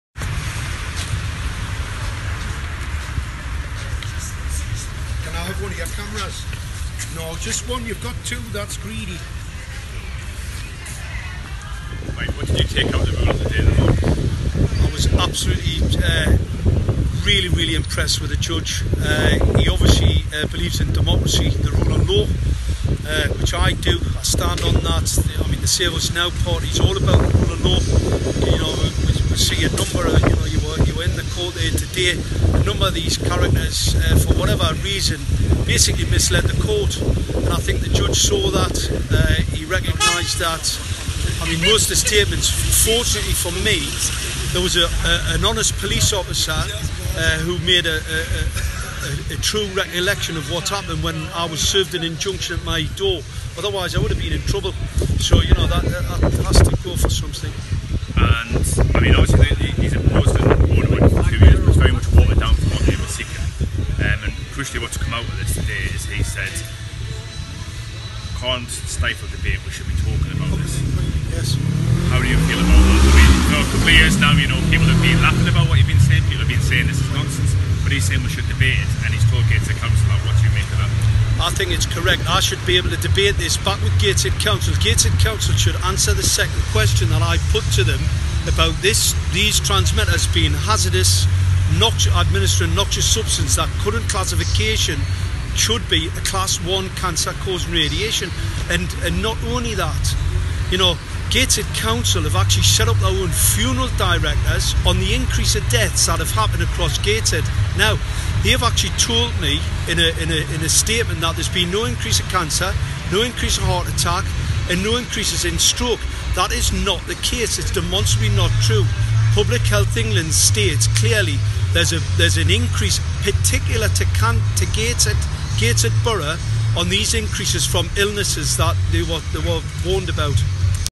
Judge REFUSES to gag anti 5G street light campaigner, stating The public have a right to know
Mark Steele, a 5G campaigner, has been highlighting the dangers of a secret 5G rollout by Gateshead Council where residents are complaining of increased illness and Cancer in the affected area. There’s enough evidence to conclude the new smart 5G arrays on the top of new LED lampposts emit Class 1 Radiation frequencies and should be treated as a danger to the Public. Gateshead Council ignorantly rebutted clear evidence and created false allegations on social media posts and printed leaflets stating that Mark Steele is spreading Pseudo Science and that the arrays are not dangerous or 5G:
“Please be assured that there is no scientific basis or credible evidence for any of these scare stories about street lights causing cancer and other illnesses.”